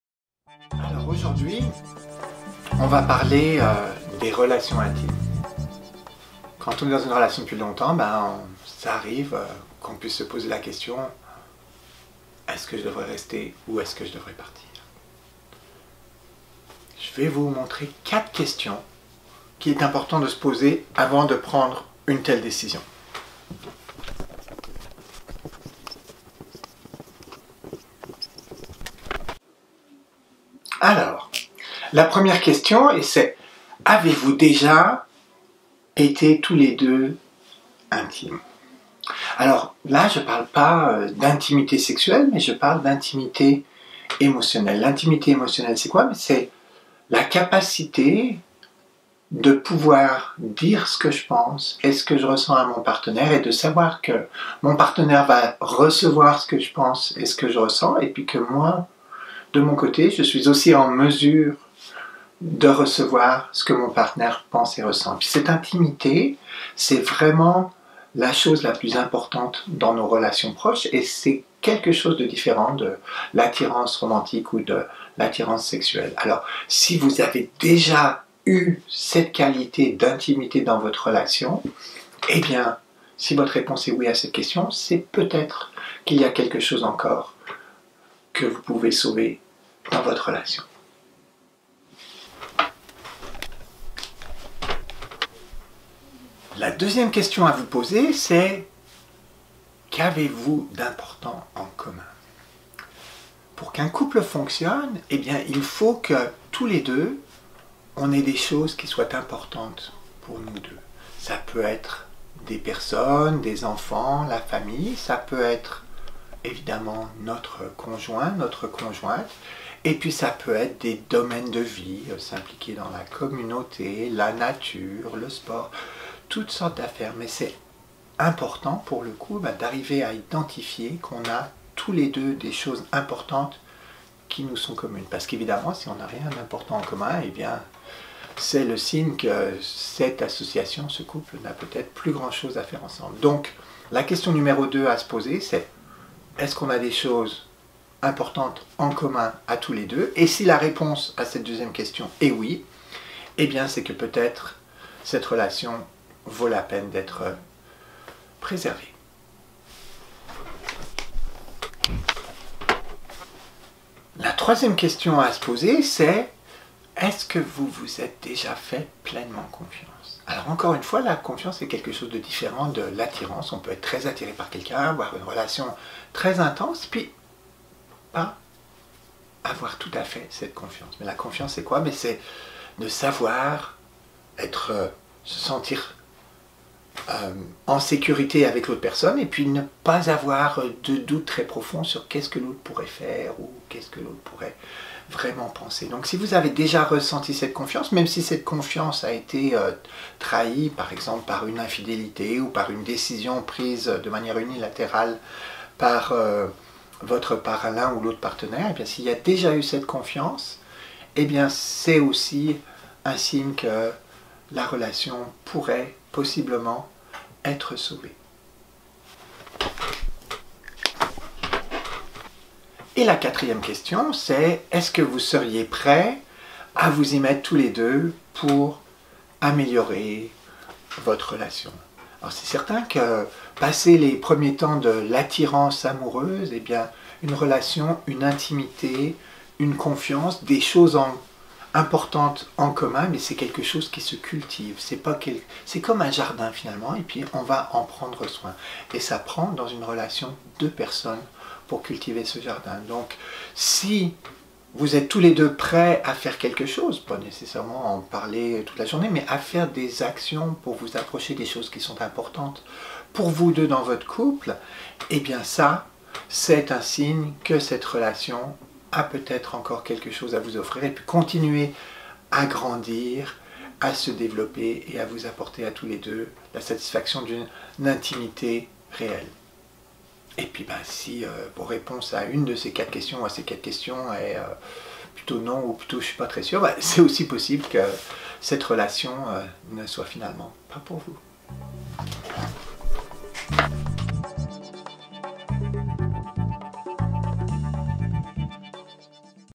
Devrions-nous rester ensemble?
4 questions à vous poser avant de prendre une décision

Vous êtes ensemble depuis un certain temps, mais ces derniers temps, il semble que la flamme a disparu. Vous avez du mal à vous connecter. Même le souvenir de vos meilleurs moments passés ensemble ne ravive plus en vous ces émotions que vous ressentiez avant. Que faire : poursuivre la relation ou y mettre un terme ?

C’est une décision importante. Voici quatre questions qui peuvent vous aider à faire votre choix et mieux reconnaitre si votre relation arrive à son terme ou si elle mérite que vous luttiez pour elle.

1. Avez déjà été intimes ?
2. Qu’avez-vous d’important en commun ?
3. Vous-êtes vous jamais fait pleinement confiance ?
4. Seriez-vous tous les deux prêts à vous y mettre ?